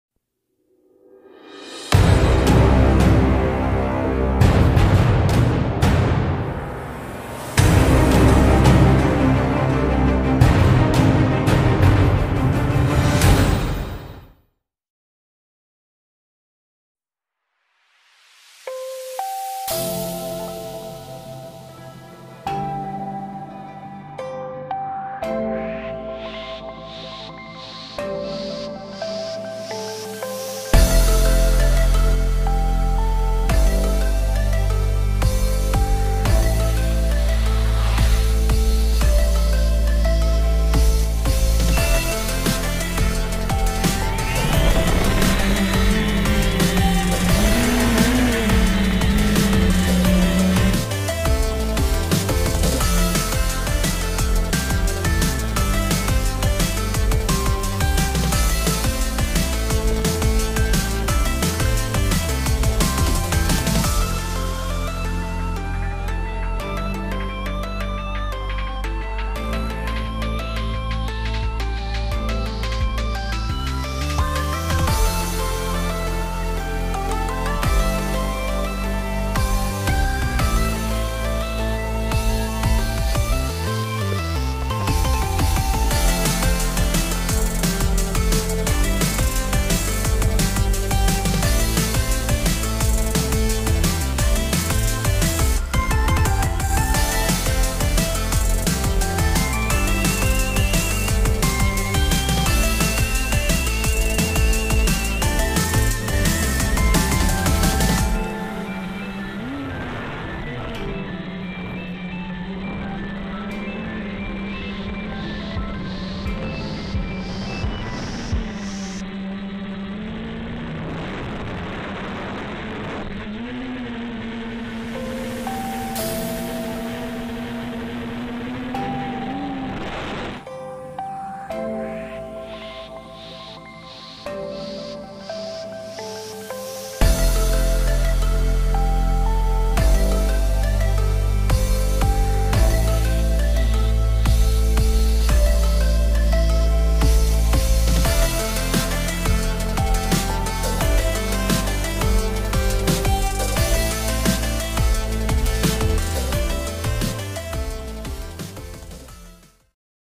Made a 450 size TBS DISCOVERY in to a FPV racing drone
We rebuilt our 450 size TBS DISCOVERY frame in to a fpv racing drone.the parts we have used are am sharing here
Frame : Team black sheep discovery
Motors : Emax 950 kv
Esc : dys 30 A
Propellers : 8045
Flight controller : naze 32
Battery : infinity 1500 mAH 4s